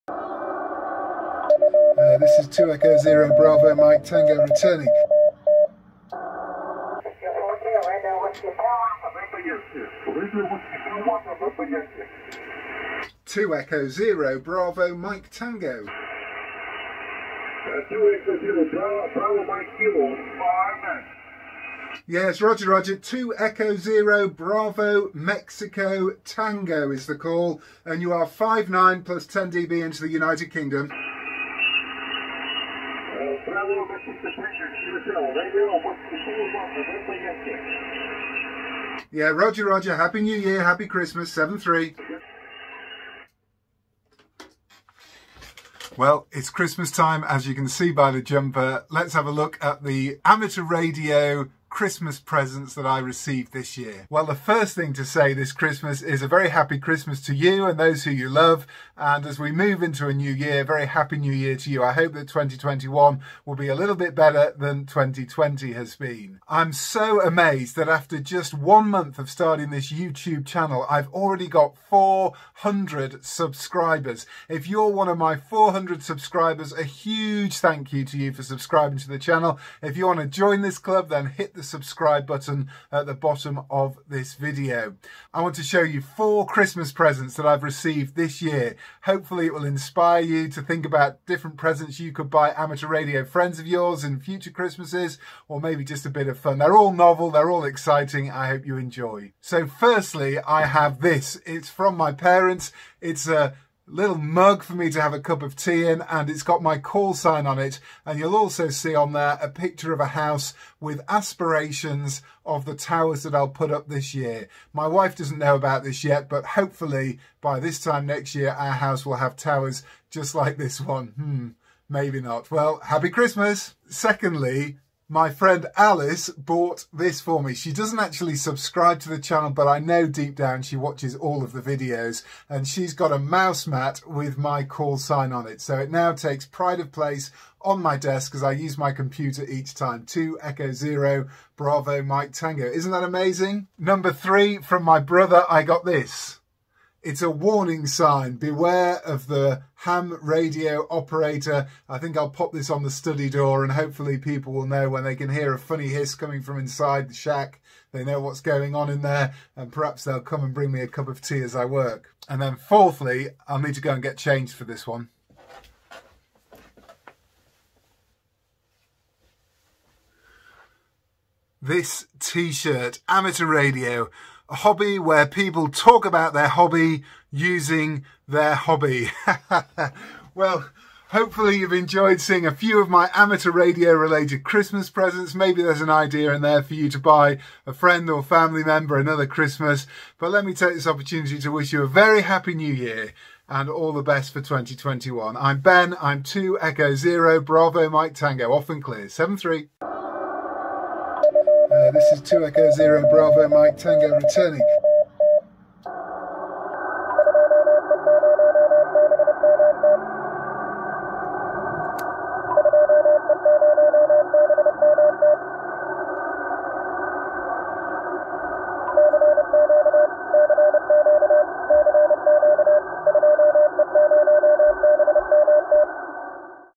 Christmas Presents for a Radio Ham Amateur Radio operator gift ideas 2E0BMT Happy New Year!
Just a bit of fun, here is a 'show and tell' of the four Christmas Presents I received this year with an Amateur Radio theme. I hope they will be fun ideas for others to buy friends and family members in the future.
73 de 2E0BMT